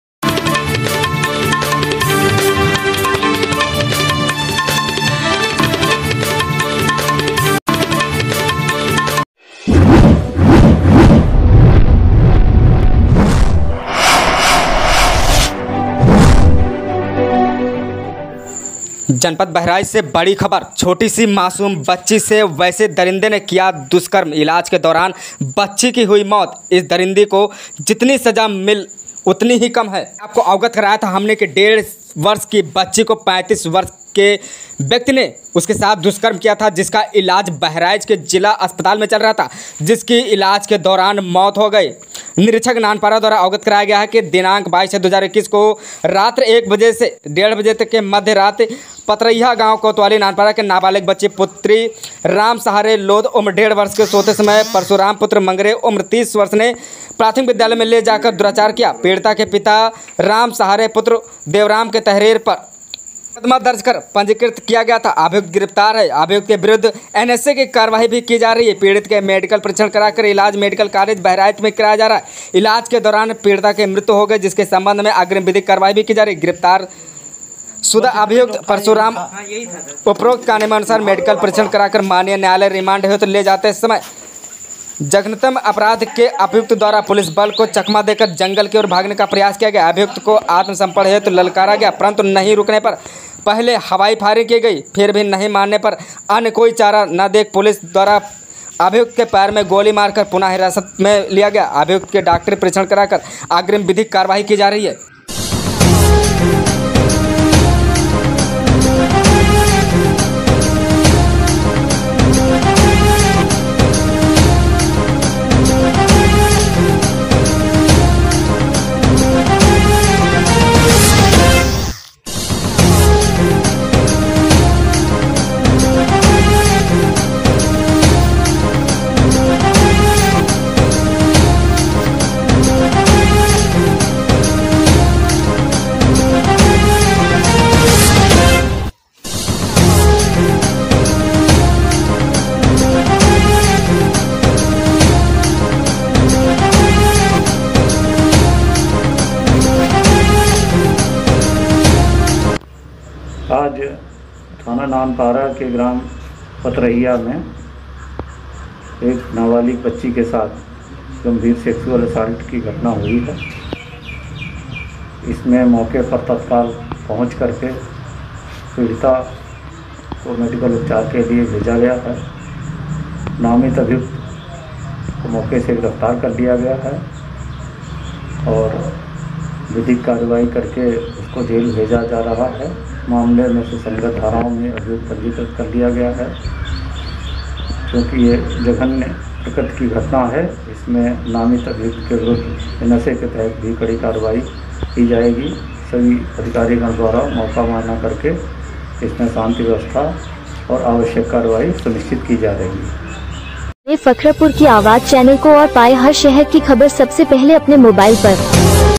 ब्रेकिंग न्यूज़ बहराइच यूपी इस वक्त की बडी खबरबहइचे ानवता को शर्मसार करने वाली घटना आयी सामने,,,,,
ब्रेकिंग न्यूज़ बहराइच यूपी

बहराइच से इस वक्त की बडी खबर

बहराइच से मानवता को शर्मसार करने वाली घटना आयी सामने,,,,,

डेढ़ वर्षीय मासूम के साथ दुष्कर्म, इलाज के दौरान हुई मौत,,,,,,

35 वर्षीय युवक ने गांव की ही लगभग डेढ़ वर्षीय मासूम बालिका के साथ दुष्कर्म की घटना को दिया अंजाम,,,,,

मासूम बालिका के परिजनों ने  सामुदायिक स्वास्थ्य केंद्र नानपारा में कराया था भर्ती,,,,,,

डॉक्टरों ने मासूम की हालत को गंभीर देखते हुए मेडिकल कॉलेज किया रेफर,,,,

बहराइच मेडिकल कालेज मे इलाज के दौरान मासूम ने तोड़ा दम,,,,

ग्रामीणों ने आरोपी दुष्कर्मी युवक को पीट पीटकर किया अधमरा,,,,

मेडिकल कॉलेज मे आरोपी का चल रहा है इलाज,,,,,

अभियुक्त के विरुद्ध होगी NSA की कार्यवाही,,,,,

एसपी सुजाता सिंह ने मामले का संज्ञान लेते हुए किया घटना स्थल का दौरा,,,,,

कोतवाली नानपारा के क्षेत्र के पतरहिया गांव का मामला,,,,,

बाइट अशोक कुमार एएसपी ग्रामीण बहराइच